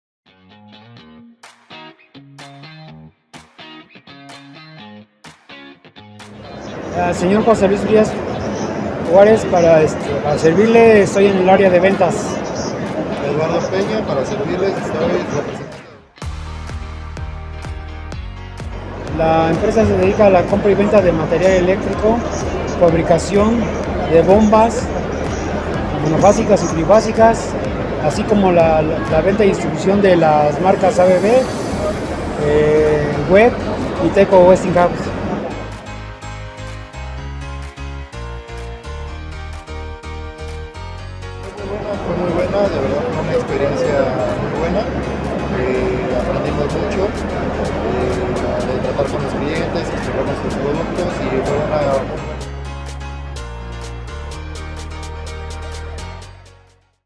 Testimonio Expo Eléctrica Internacional 2023
Escucha lo que Turbolight dijo sobre  Expo Eléctrica Internacional 2023. 🌍🤝🏻
¡Gracias por su participación! Nos vemos en 2024.